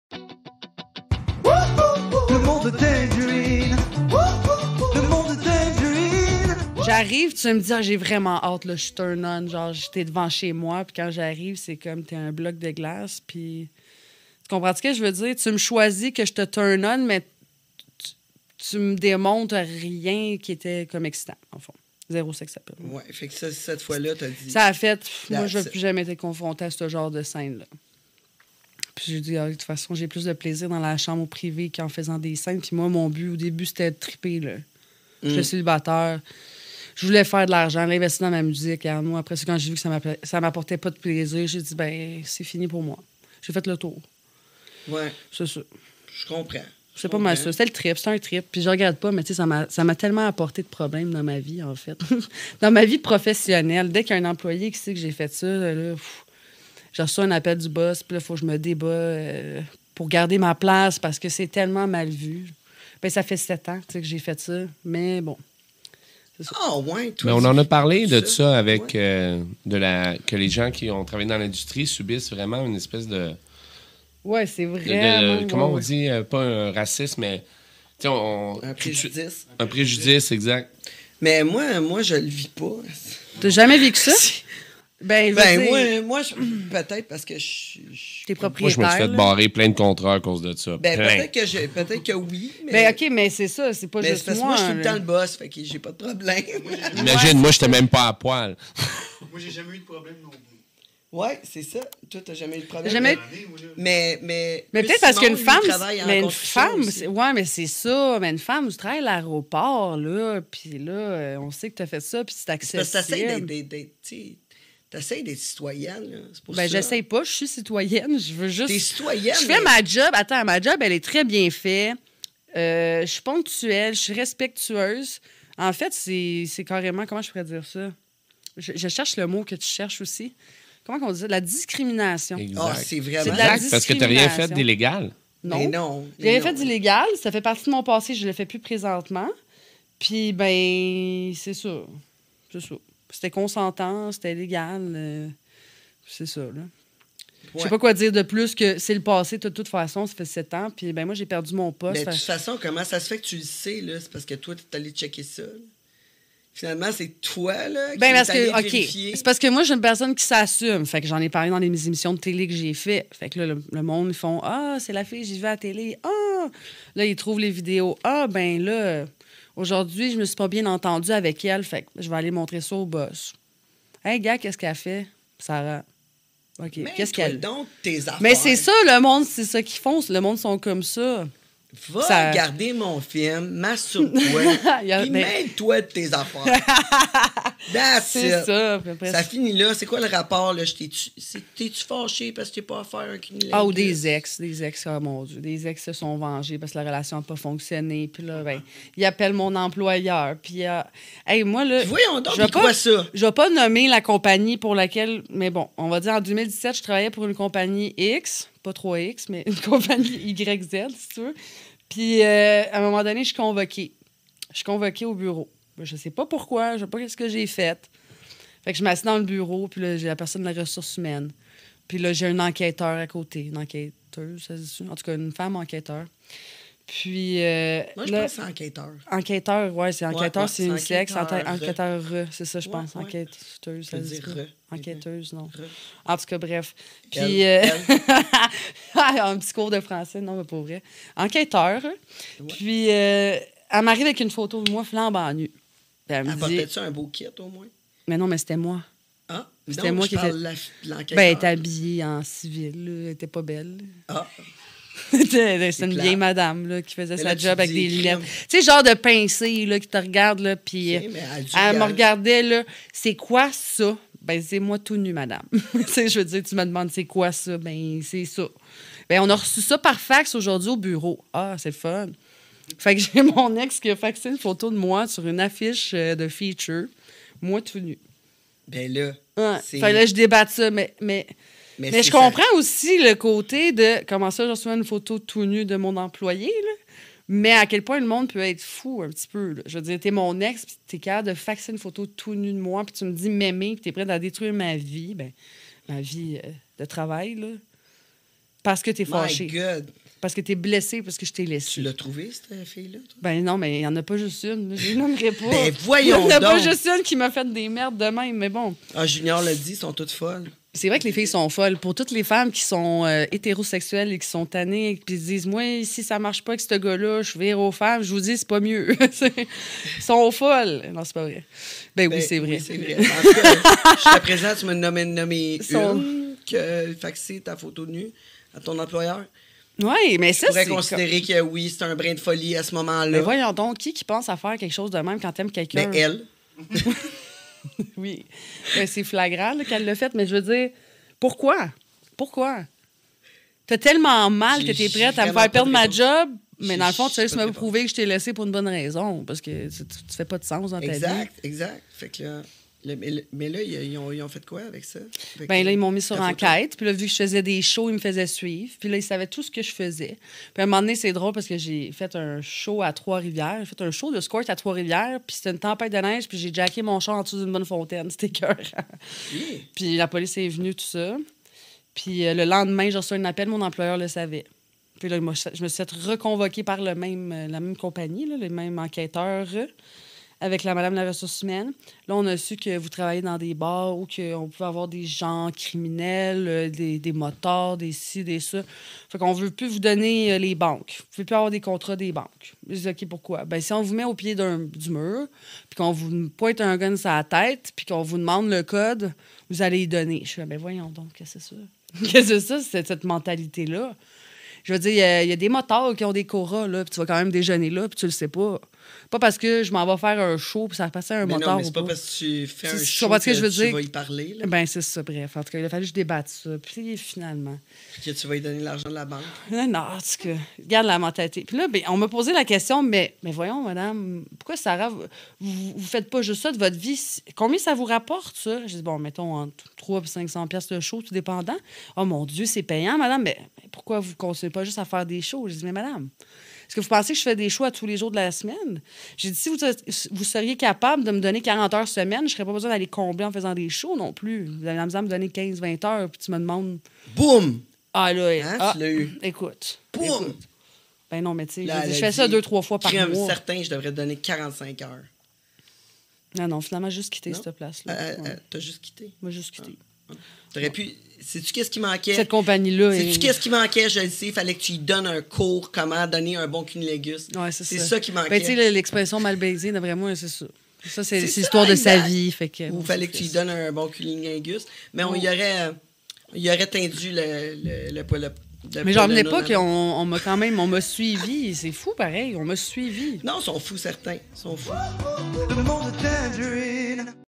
"C'est quoi ça? C'est moi toute nue!" - SD50 - Artiste Rap / Films XXX
Nous recevons aujourd'hui au podcast, la rappeuse SD50 @sd50ketchup qui a financé sa musique avec des films pour adultes. Elle nous raconte des histoires inédites sur son parcours et le milieu musical.